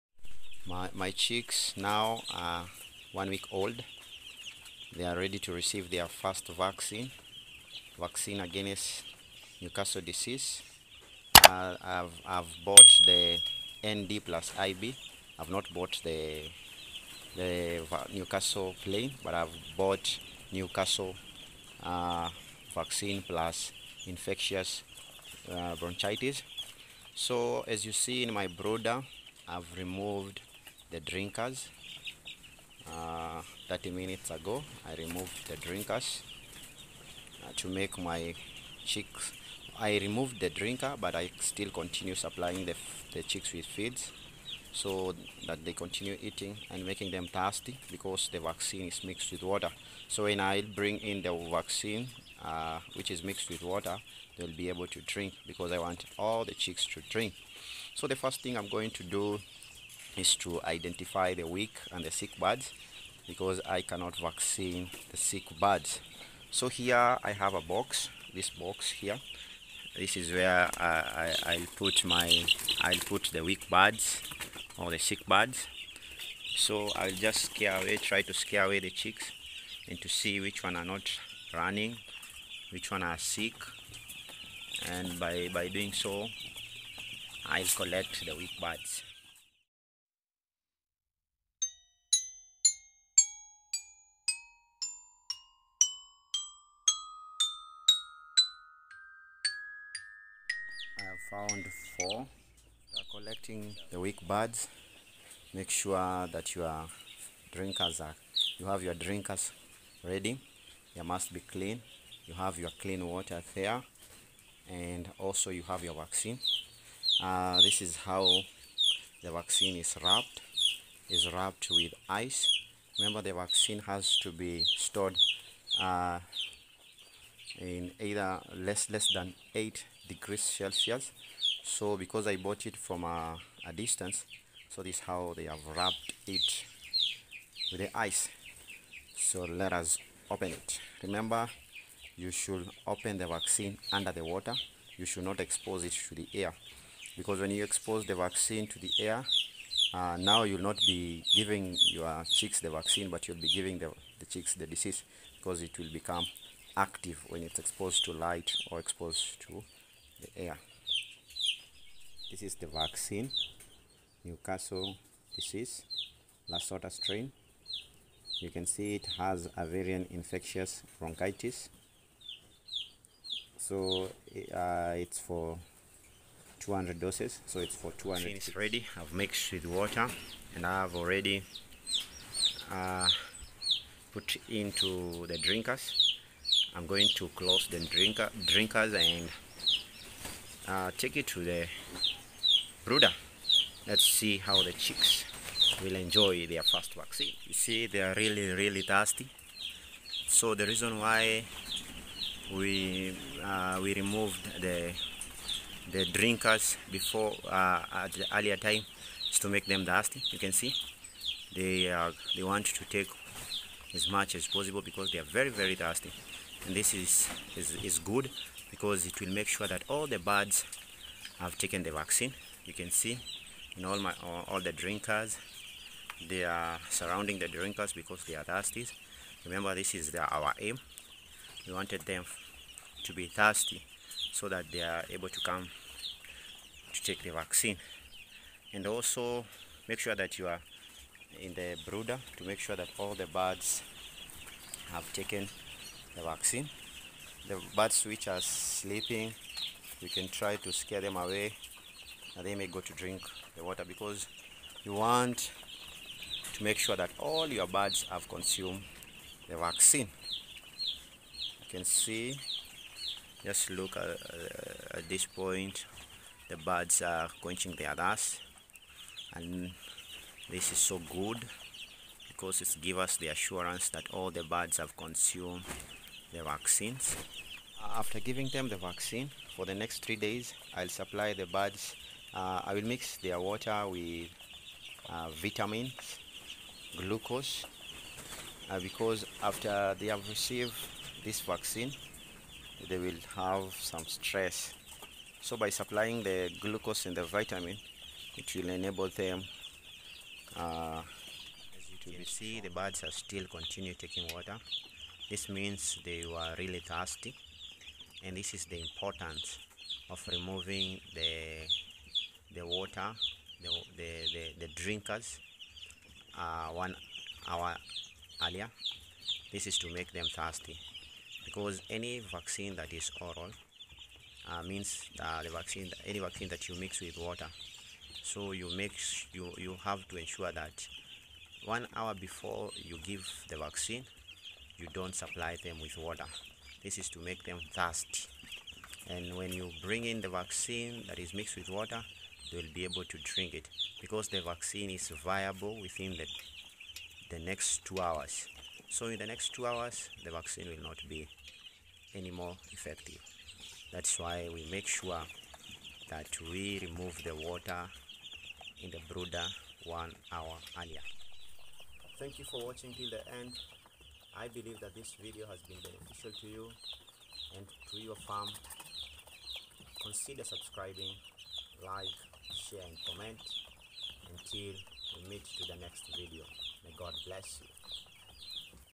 mk smart farm/vaccinating my chicks against Newcastle disease
In this i video I have vaccinated my chicks against Newcastle disease.
I have also demonstrate the procedure of ensuring that all your chicks have taken in the vaccines.The vaccination is carried out within the 5-7 days.